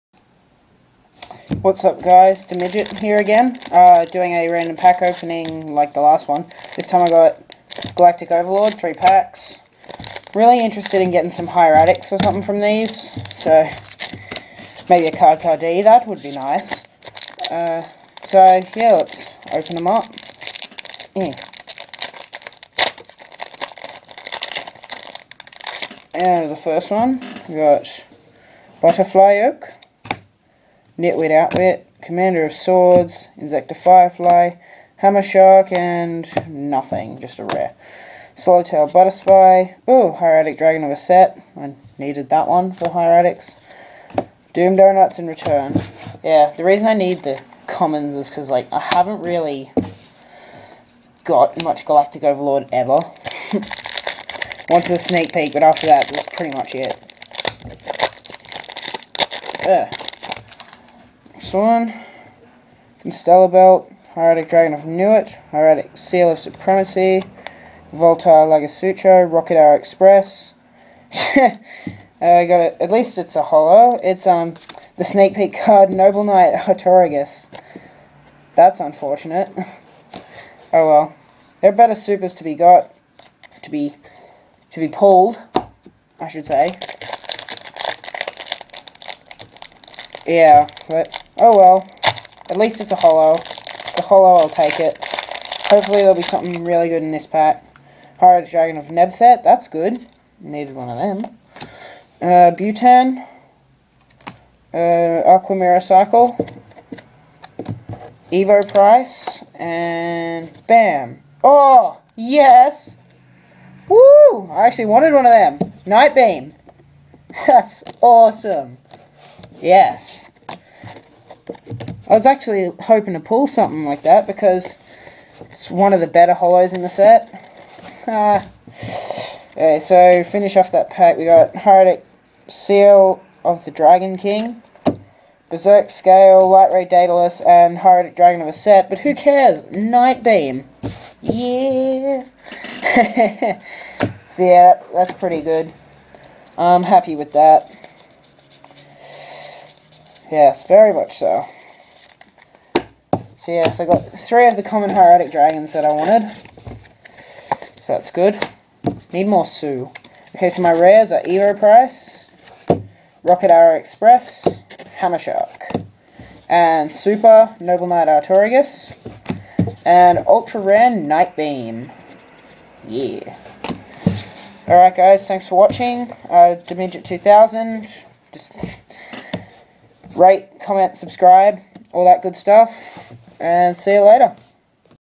Yugioh Galactic Overlord Pack Opening
Me opening 3 packs of Galactic Overlord, hope you enjoy :)